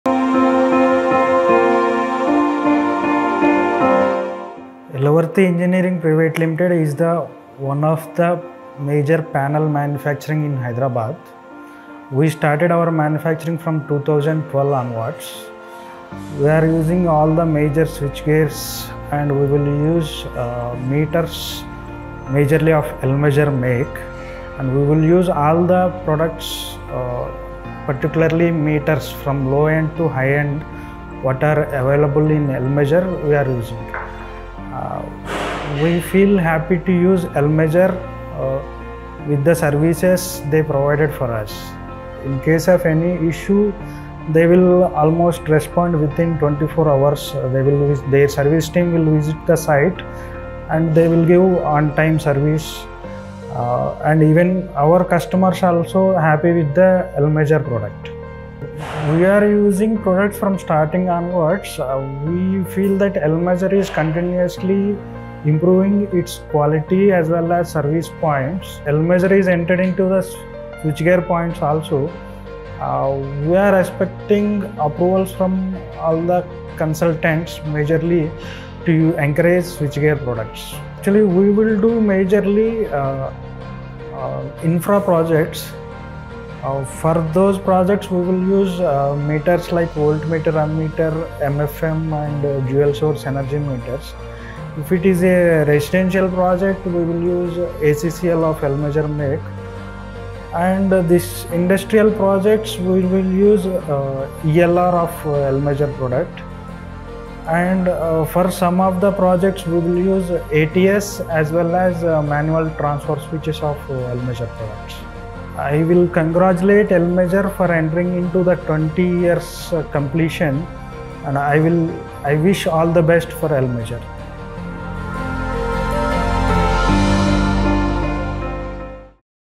Mr. GV Suresh of Yalavarti Engineering Pvt Ltd | 20 Years of Elmeasure | Customer testimonial
In this video, Mr. GV Suresh, from Yalavarti Engineering Pvt Ltd, shares his experience using Elmeasure’s energy meters and expresses his satisfaction with our reliable products. He highlights the exceptional support and service his team has received, contributing to their smooth operations. Mr. Suresh also talks about Elmeasure’s exciting entry into the switchgear market and his hopes for continued innovation. Lastly, he extends his warm wishes to Elmeasure on reaching the milestone of 20 successful years in the industry.